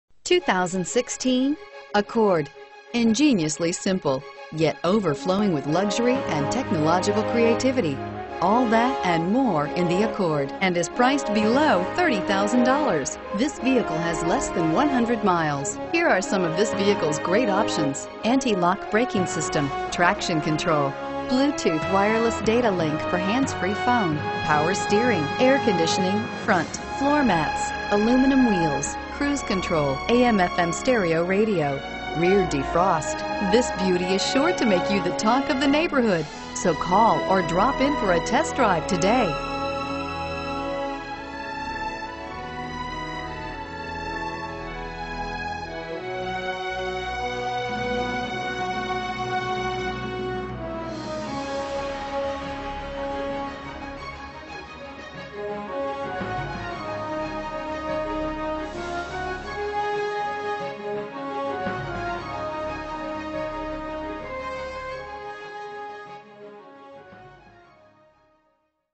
2016 Honda Accord Sedan McDonough GA 17301
2016 Honda Accord Sedan  - $25785

Sons Honda
105 Sons Drive

Looking for the right 4 Door Car? Today could be your lucky day. With 6 miles, this Obsidian Blue Pearl, 2016 Honda Accord Sedan equipped with Automatic transmission could be yours. Request more information and set up a test drive right away.